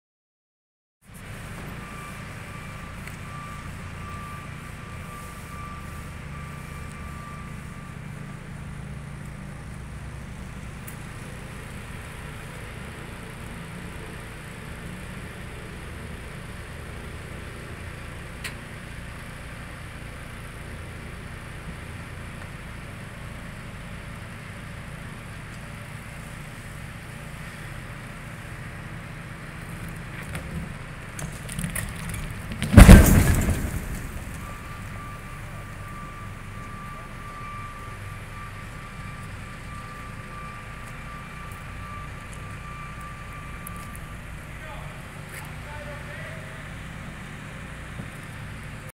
ICC wall opening getting dropped
dropping a 20x14 wall opening in Redlands CA